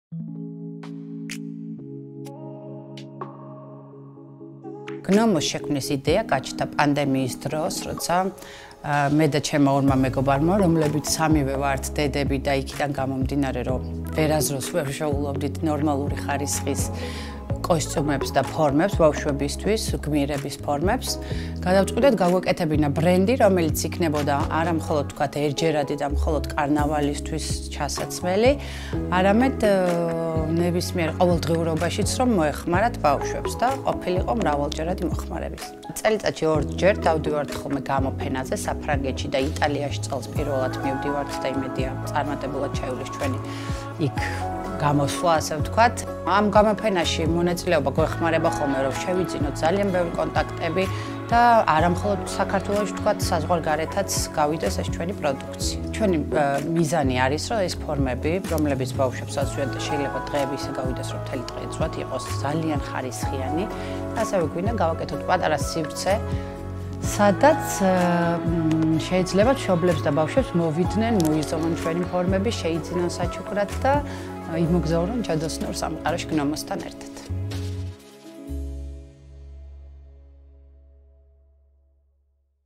მეგზურსაც გააჩნია - გნომო
გნომოს შექმნის იდეა პანდემიის დროს გაჩნდა. სამი მეგობარი ერთმა მიზეზმა, თავიანთი შვილებისთვის გმირების ხარისხიანი კოსტიუმების დეფიციტმა გააერთიანა.

,,გადავწყვიტეთ გაგვეკეთებინა ბრენდი, რომელიც იქნებოდა არამხოლოდ ერთჯერადი და კარნავალისთვის, არამედ ბავშვები ყოველდღიურობაშიც გამოიყენებდნენ.”

გნომოს მიზანია შექმნას სივრცე, სადაც მშობლებს და ბავშვებს საშუალება ექნებათ მივიდნენ, მოიზომონ ფორმები, შეიძინონ საჩუქრად და იმოგზაურონ ჯადოსნურ სამყაროში.

სოკარი - მეგზურსაც გააჩნია!